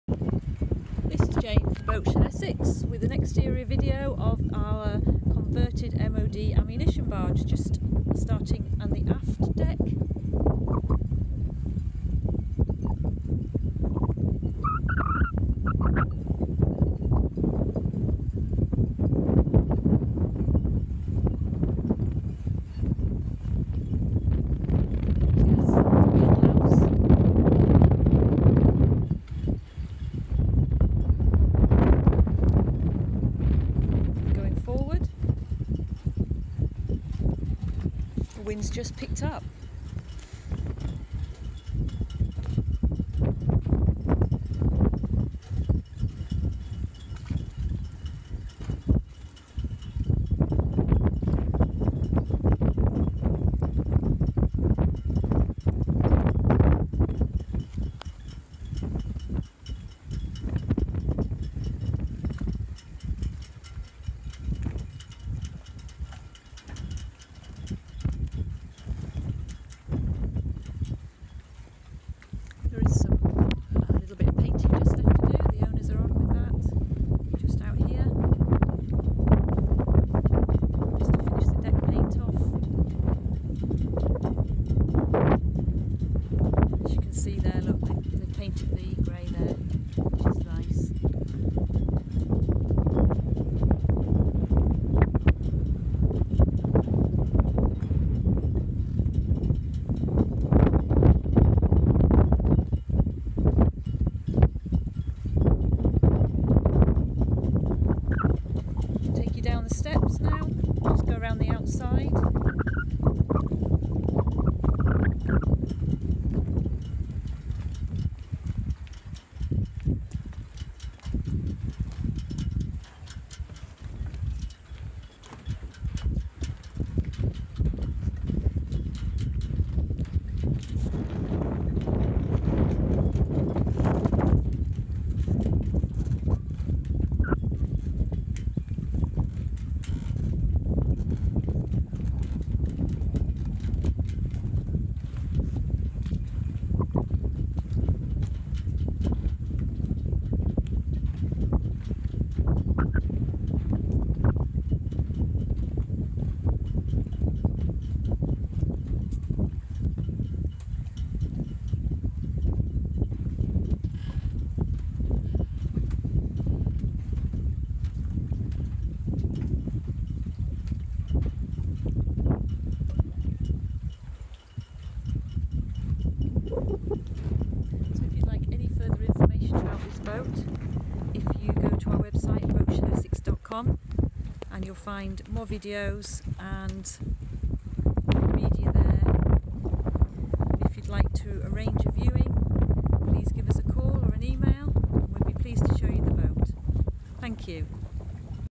Barge Converted Ammunitions Barge - Boatshed - Boat Ref#311164
Barge Converted Ammunitions Barge for sale with Boatshed Essex Photos and video taken by Boatshed Essex